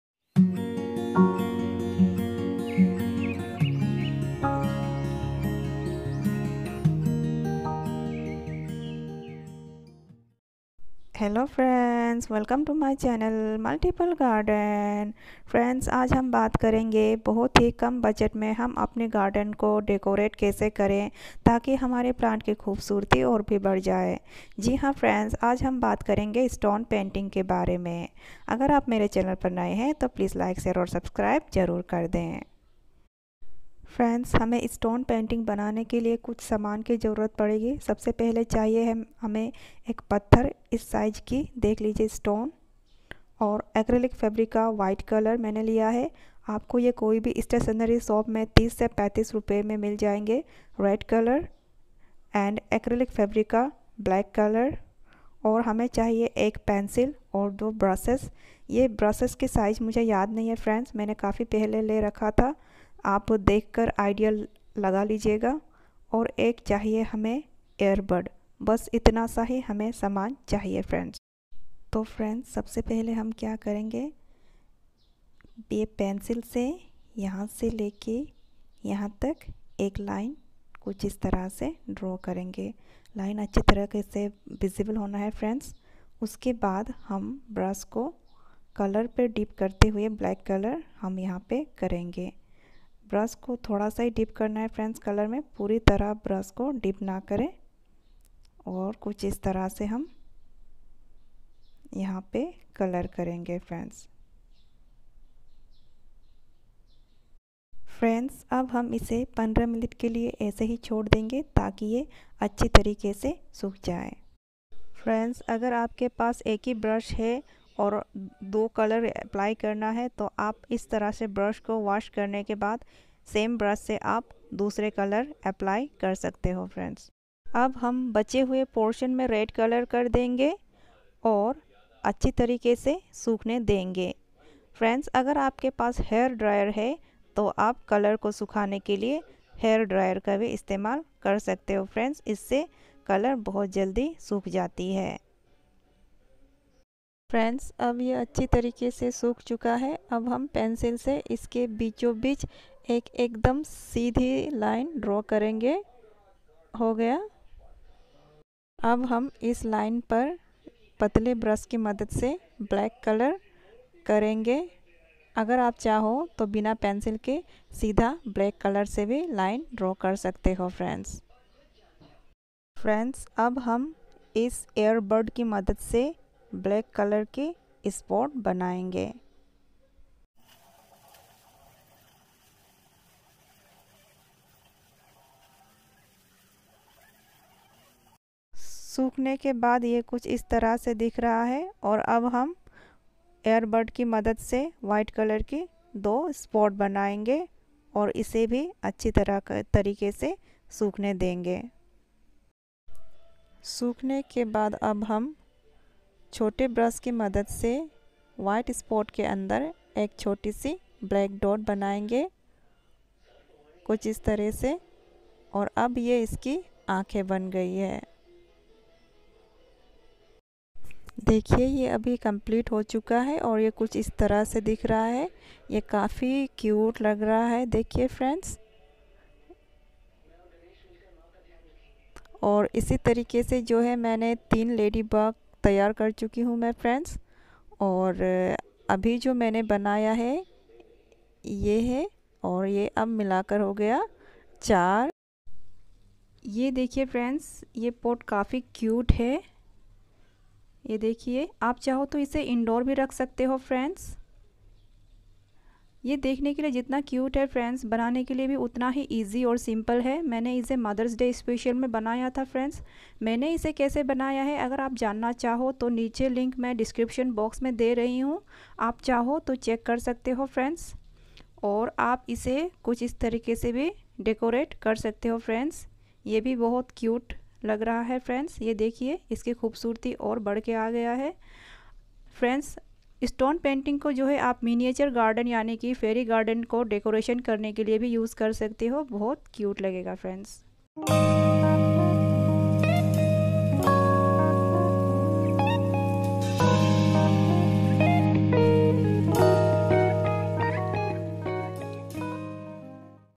Decorate your garden with low budget// lady bug stone painting.
in this video you can see the easiest way to making stone painting